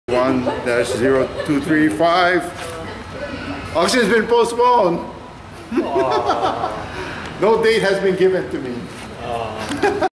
Foreclosure Auction Maui Hawaii 1014/2016 805 Pala Circle Kahului HI 96732
Foreclosure Auction in the county of Maui Hawaii at 2145 Main Street Wailuku, Hi. 96793 The following property 805 Pala Circle Kahului HI 96732 was postponed by their respective lien holders.